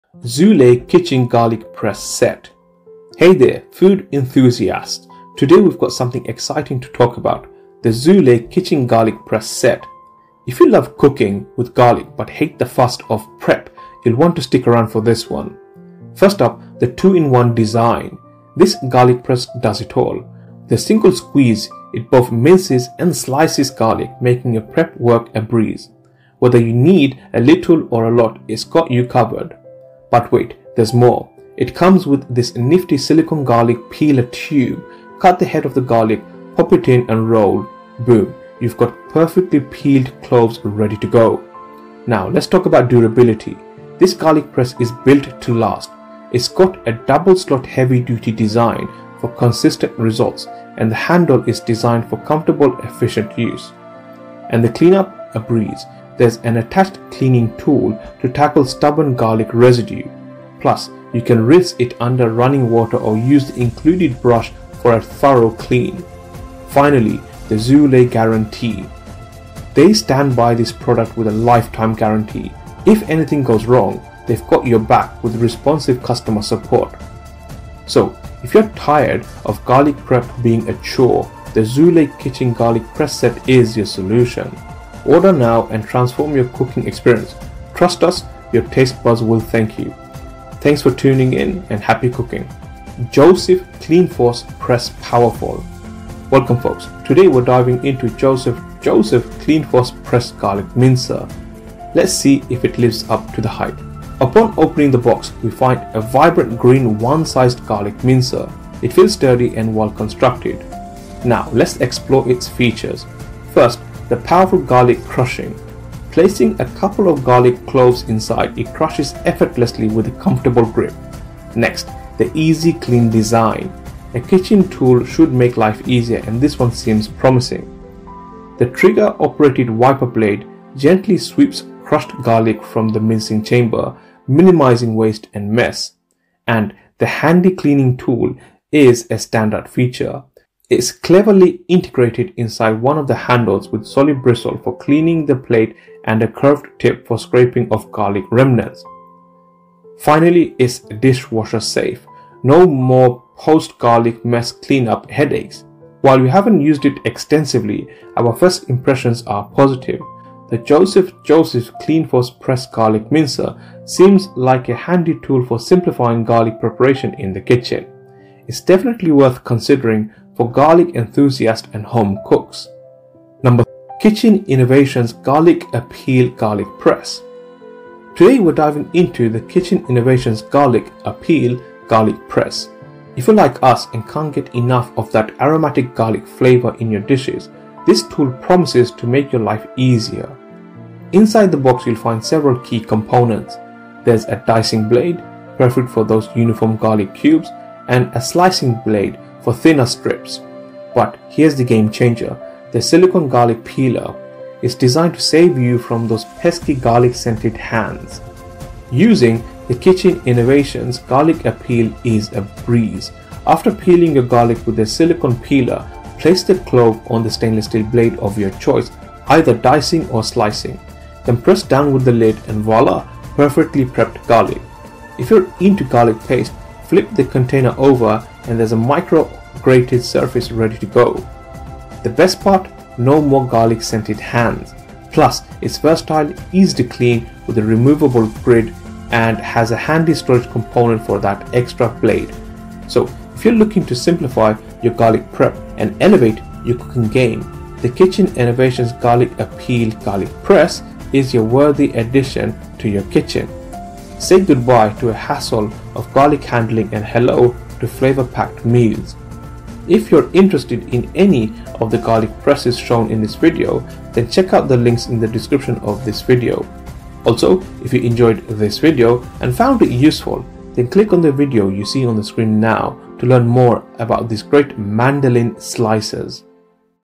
The "3 Garlic Press" Showdown! | Zulay, Joseph & Kitchen Innovations
Introducing the “3 Garlic Press” Showdown: Zulay Kitchen vs. Joseph Joseph vs. Kitchen Innovations. Garlic prep made easy with these innovative kitchen tools.

0:00 Zulay Kitchen Garlic Press Set
USA [Paid Link]

1:32 Joseph CleanForce Press Powerful
USA [Paid Link]
UK [Paid Link]

2:52 Kitchen Innovations Garlic-A-Peel Garlic Press
USA [Paid Link]
UK [Paid Link]